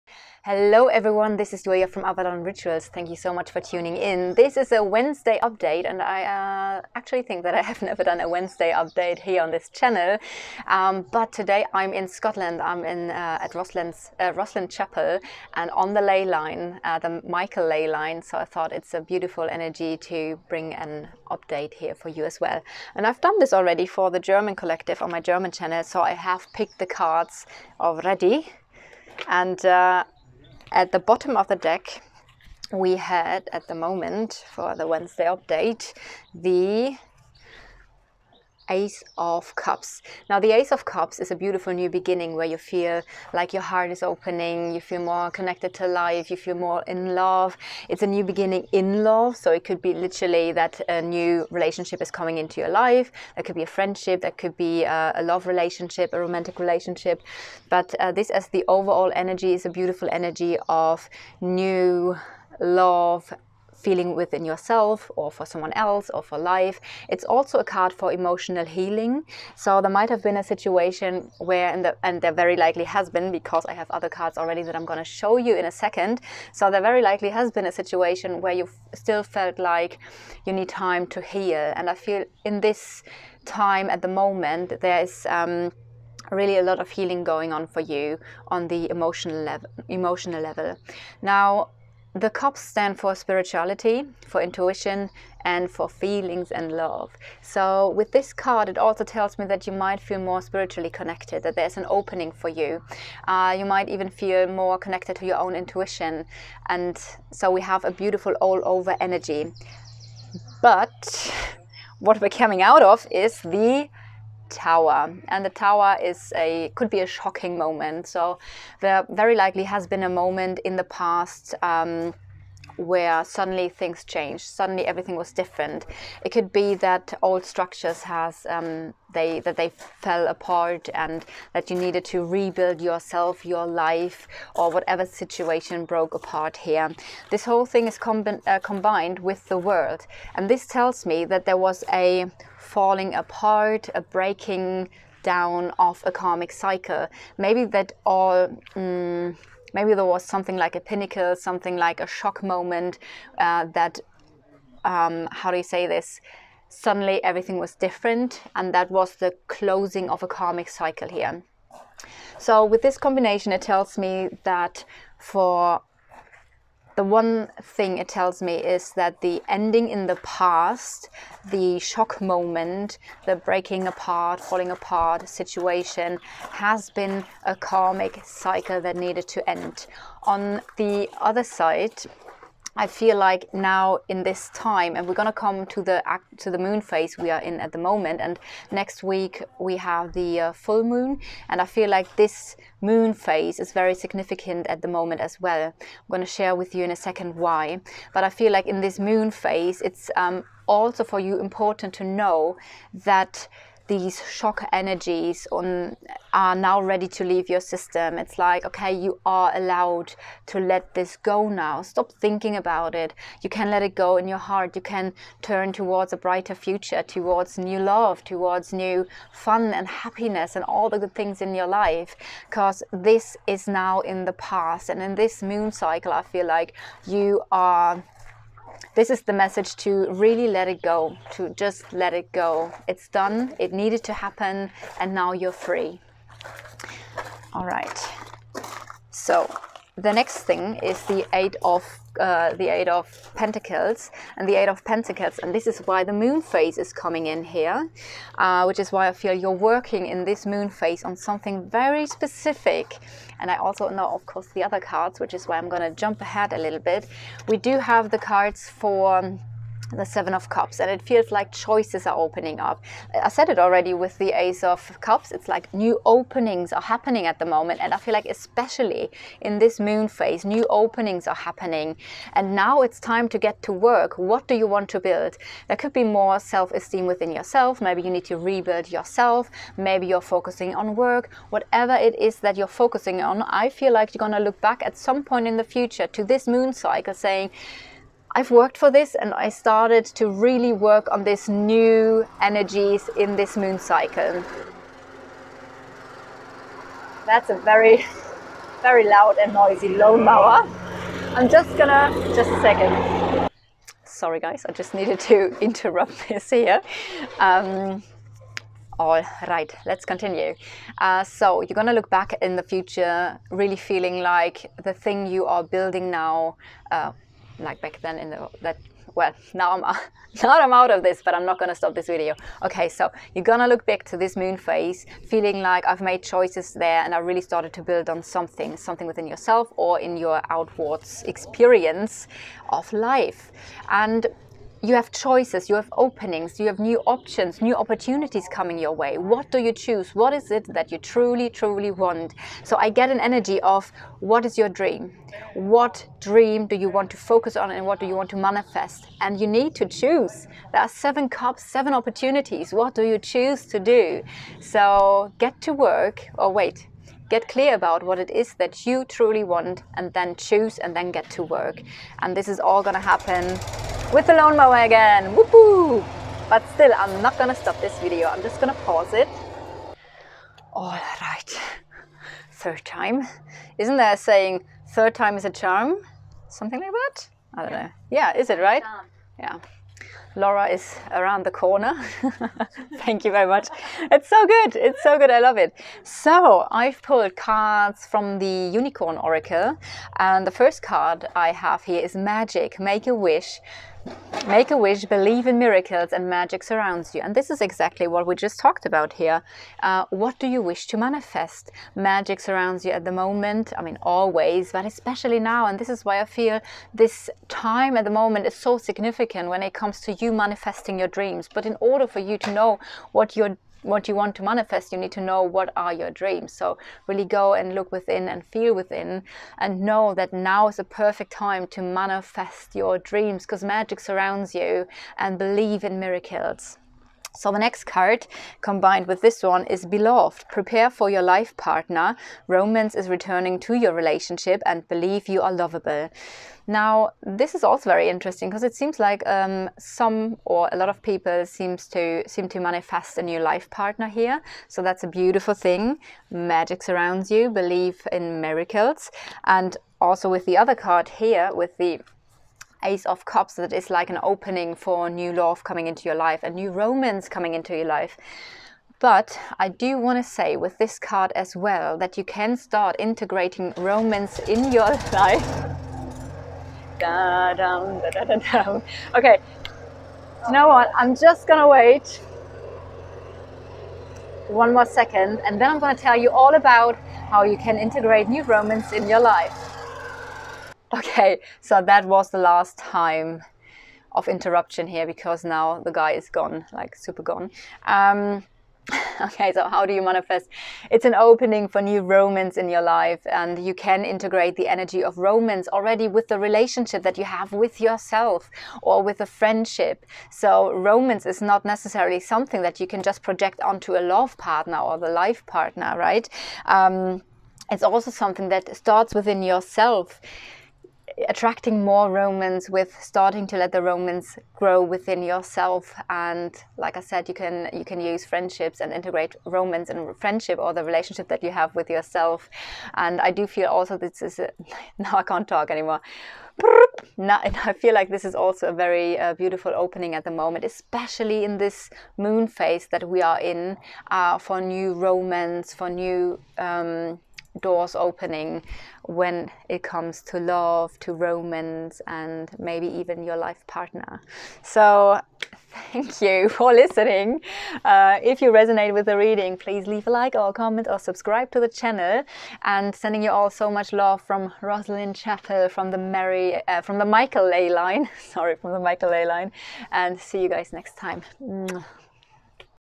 Mid week tarot | You're manifesting your dreams into reality | live from Rosslyn Chapel in Scotland
Cards: light seer tarot | the oracle of the Unicorns

My German channel @Weil du schön bist